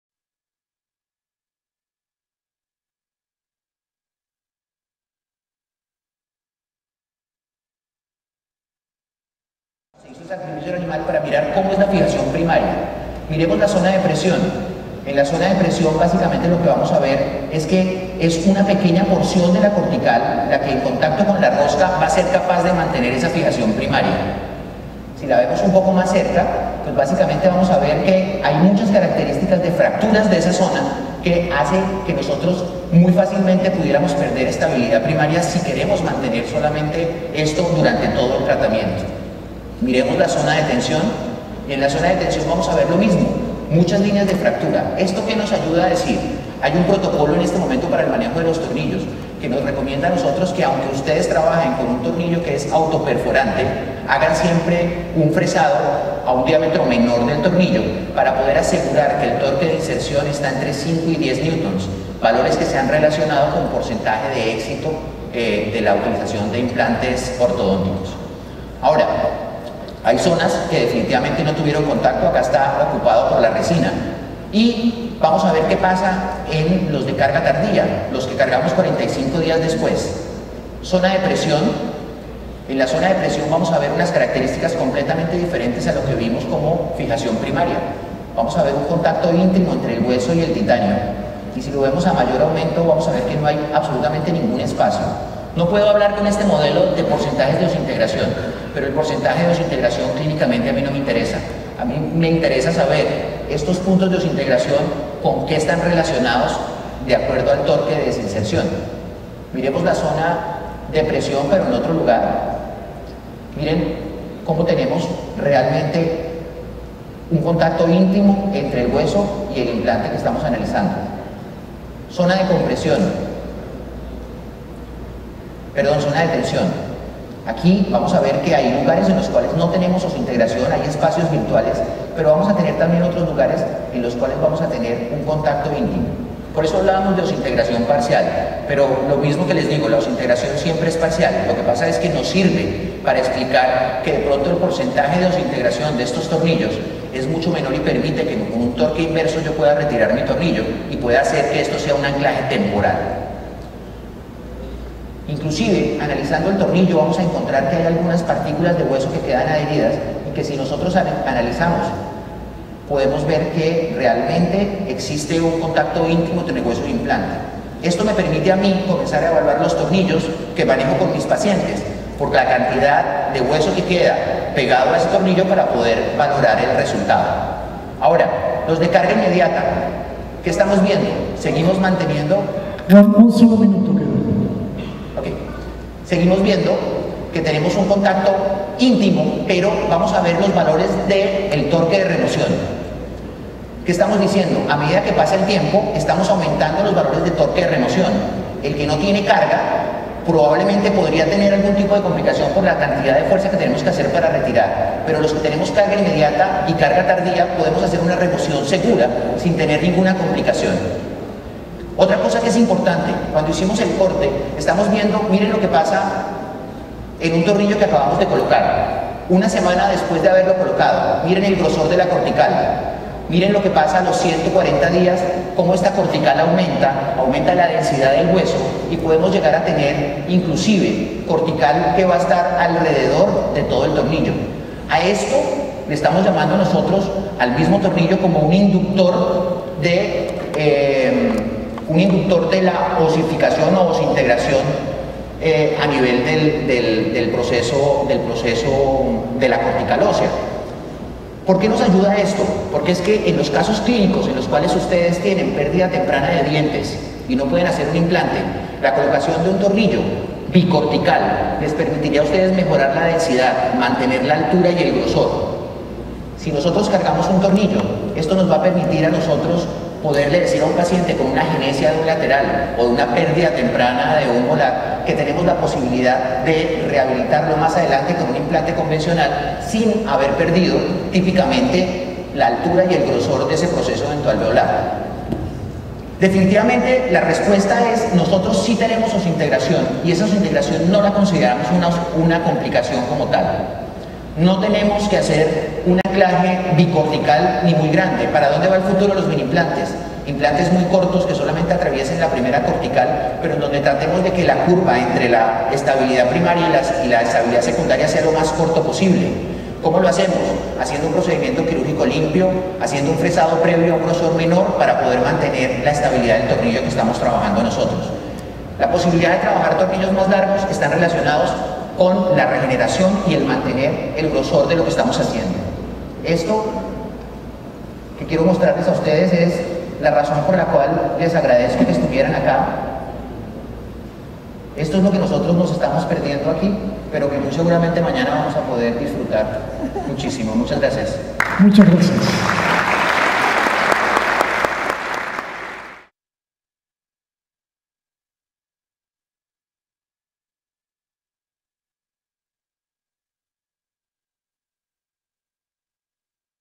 Osseointegration - A complication for orthodontic implants? Part 3
- Dr. Martin Casale Rozo raises the question - Osseointegration - A complication for orthodontic implants?
A lecture given In Cancun Mexico during the MIs-Implants annual conference. Part 3